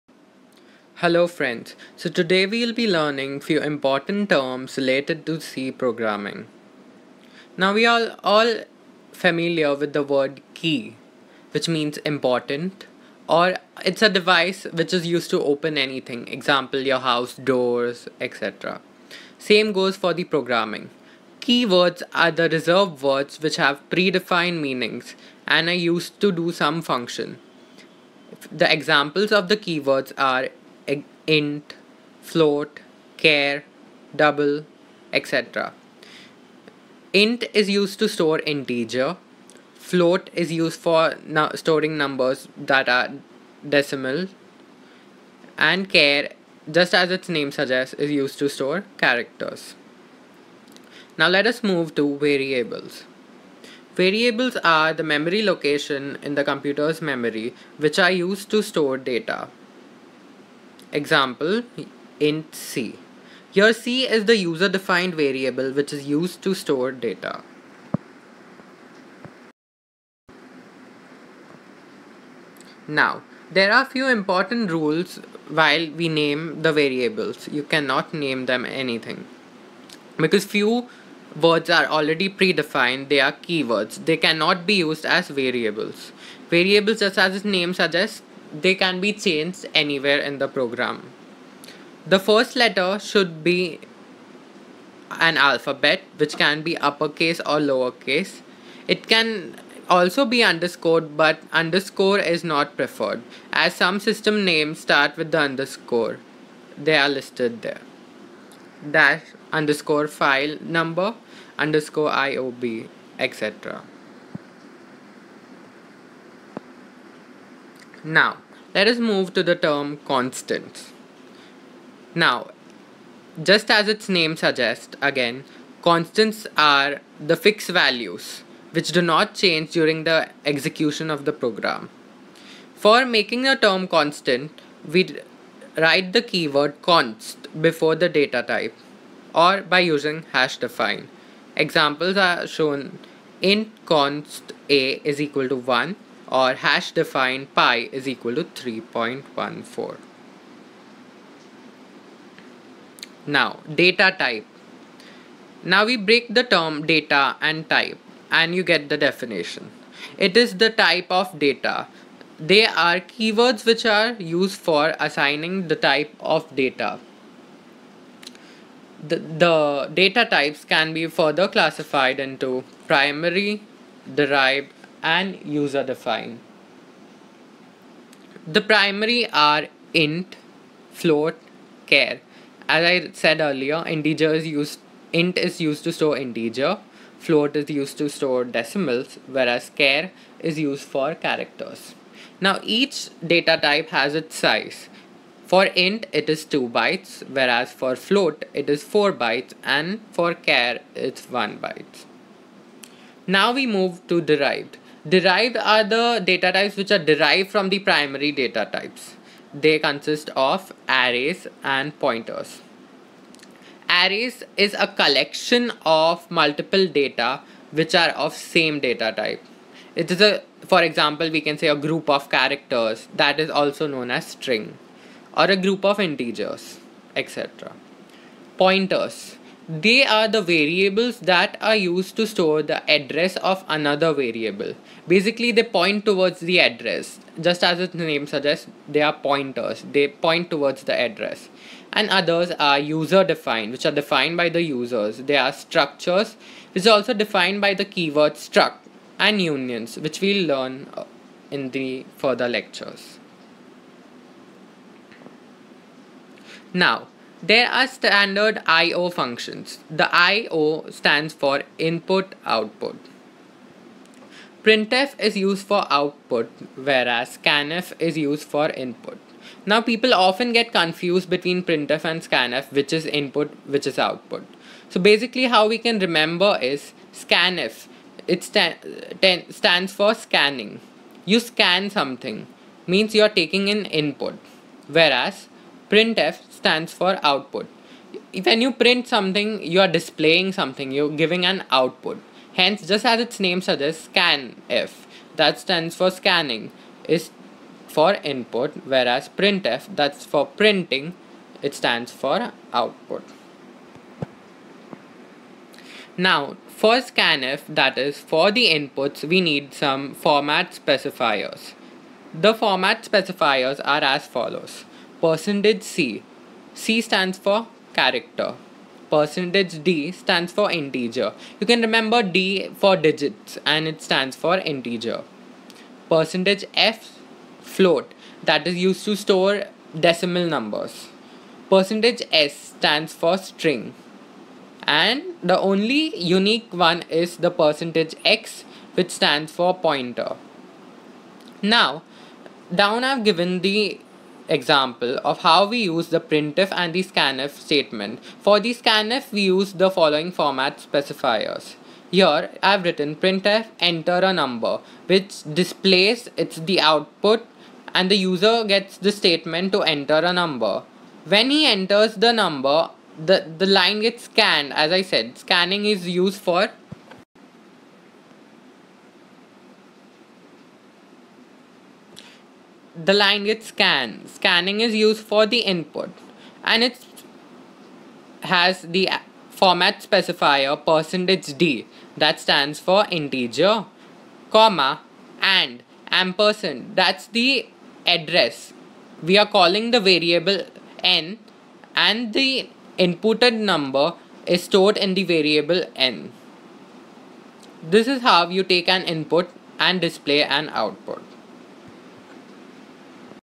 Basic Concepts of C Programming - Important Terms in C Programming Part 1
In this lesson, Hrishikesh Damania has explained about some very important terms of  C Programming.   This lesson will clear the concepts of c programming and make programming easy as possible for you.

You can watch the full list of courses and start discussions with the educator